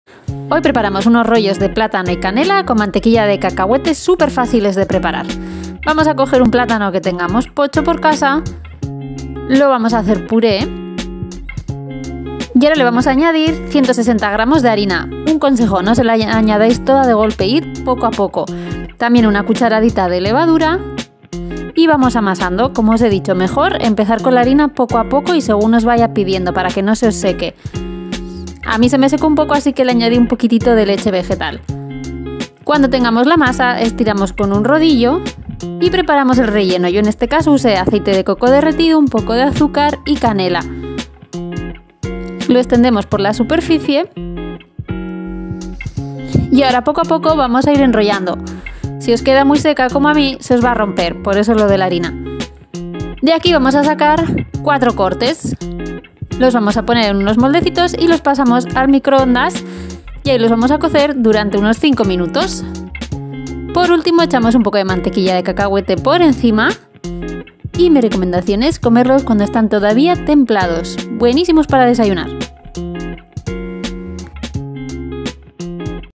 Rollos de plátano y canela con mantequilla de cacahuete
Si tenéis algún plátano pocho y antojo de dulce,  no podéis perderos esta receta. Se prepara en 5 minutos y están buenísimos!

INGREDIENTES:

- 1 plátano maduro
- 160 g de harina de avena (aprox)
- 1 cucharadita de levadura
- aceite de coco + azúcar + canela
- mantequilla de cacahuete

Para no perderos nada, seguidme en RRSS:

Facebook: Ni Blanco Ni en Botella